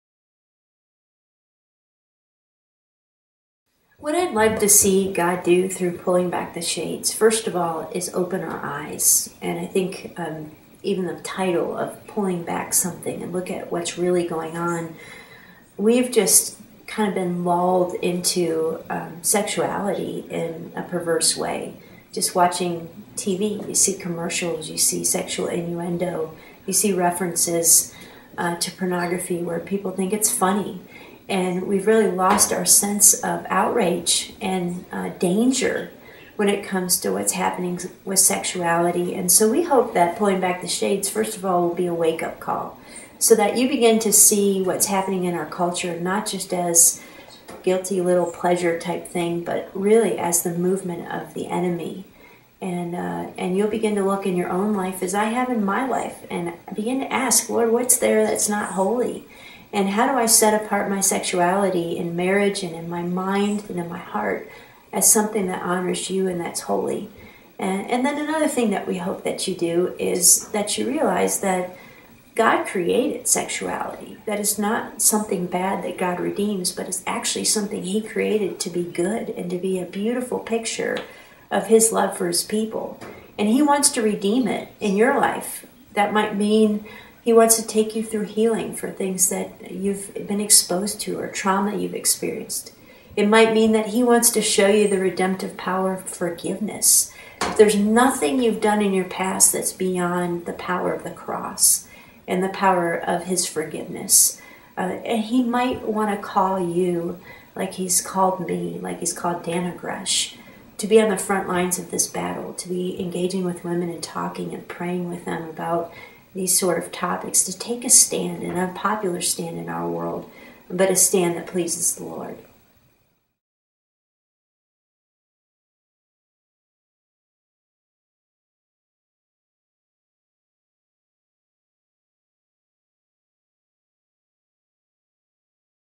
Pulling Back The Shades Affect w/Dr. Juli Slattery
Dr. Juli Slattery shares her heart for how she wants the new book, Pulling Back The Shades to minister to women.